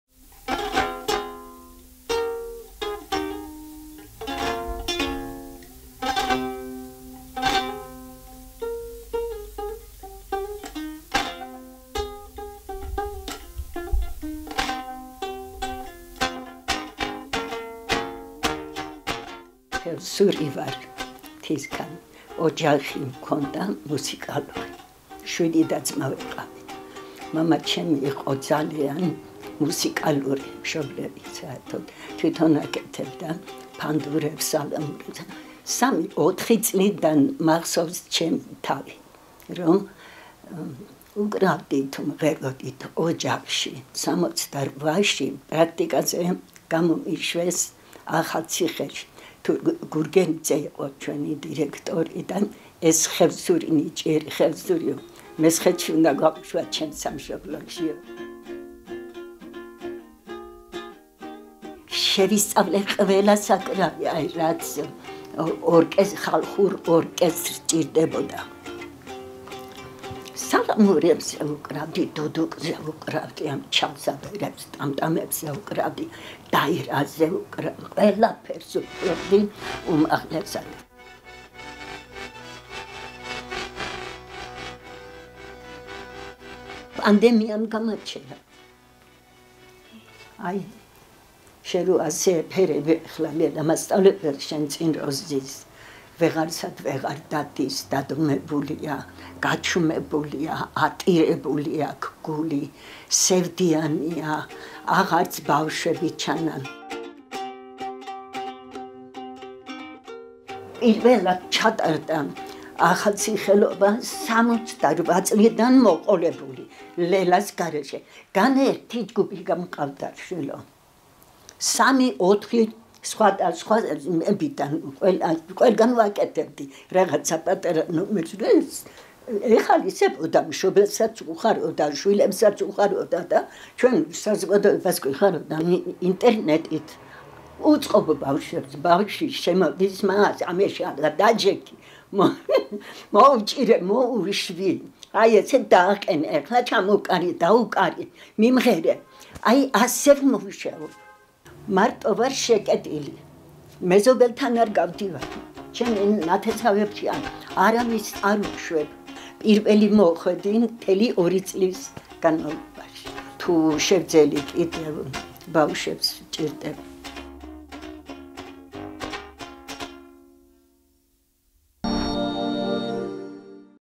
დადუმებული საკრავები - 76 წლის დირიჟორი ლელა ნაროზაული ახალციხიდან
76 წლის ლელა ნაროზაულს ახალციხეში ყველა იცნობს. ის თორმეტ მუსიკალურ ინსტრუმენტზე უკრავს და პანდემიამდე სხვადასხვა სოფლისა თუ ქალაქის მუსიკალურ და სამრევლო სკოლებში ასწავლიდა.